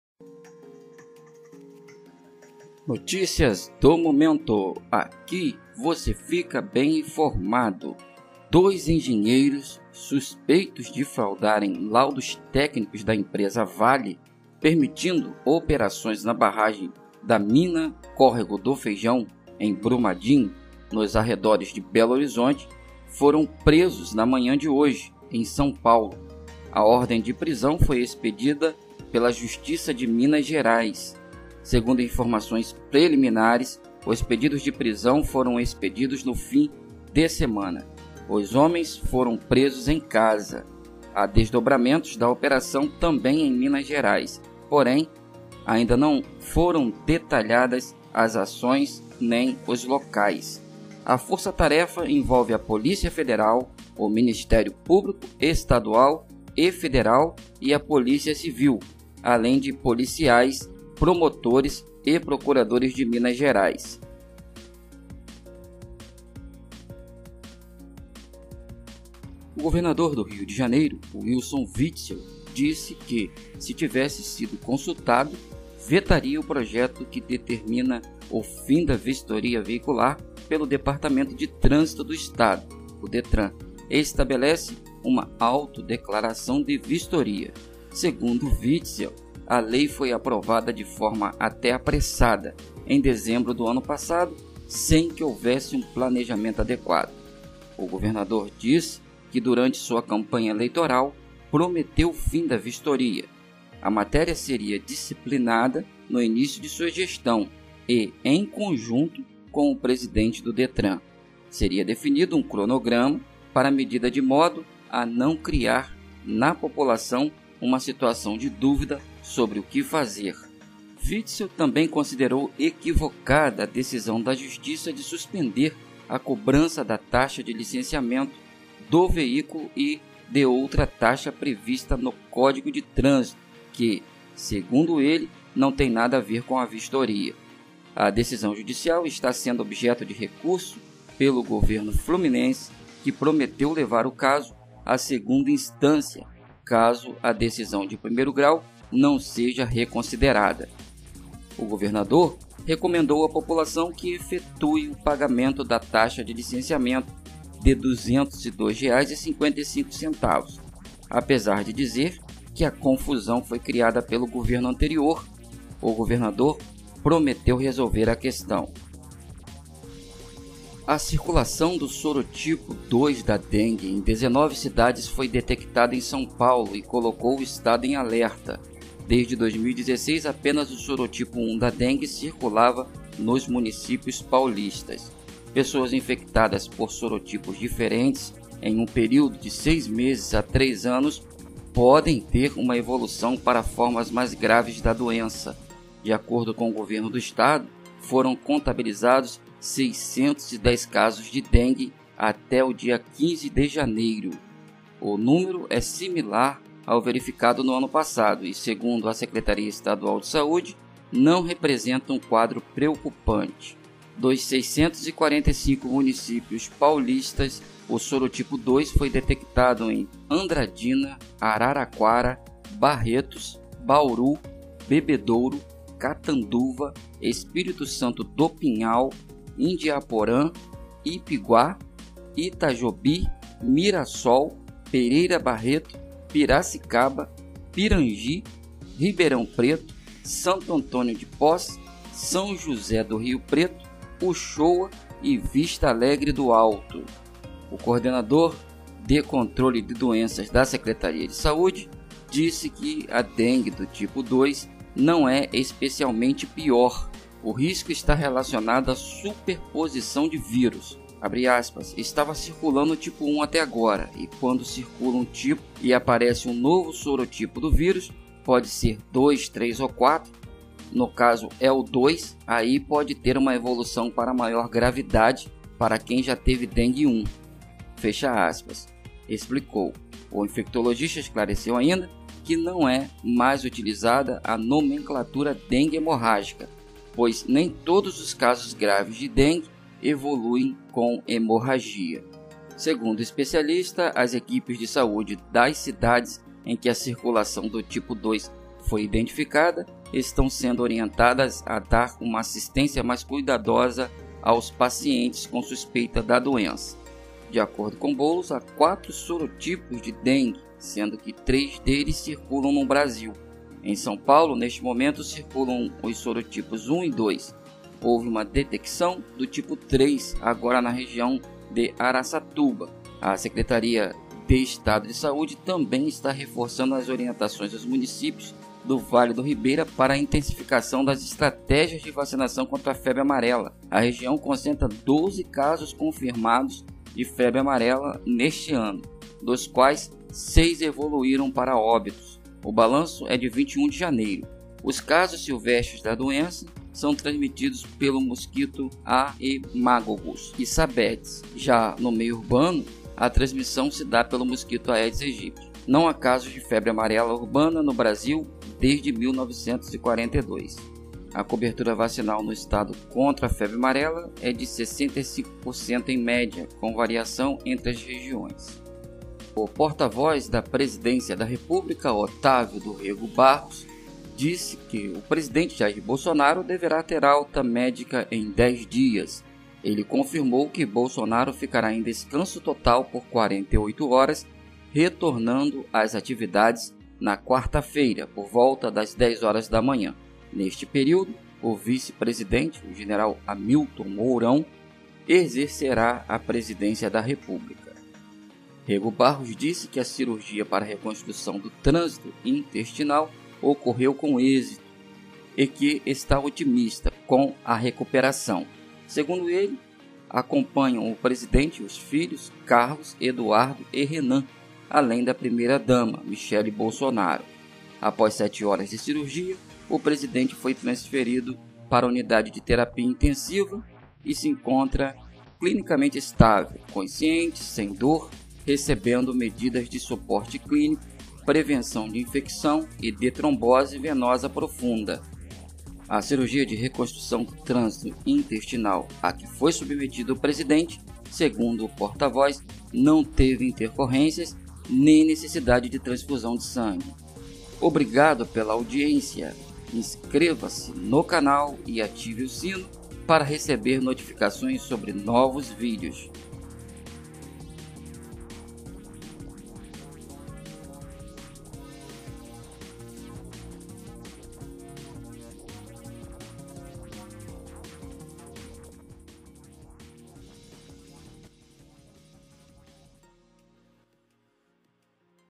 Engenheiros suspeitos de fraudes em Brumadinho; Bolsonaro deverá ter alta médica em dez dias.
Engenheiros suspeitos de fraudes em Brumadinho; Bolsonaro deverá ter alta médica em dez dias. Outras notícias...

Quer ajudar o canal? Faça uma doação!

Banco Santander
Agência: 1471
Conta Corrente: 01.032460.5
Washington Luiz Teixeira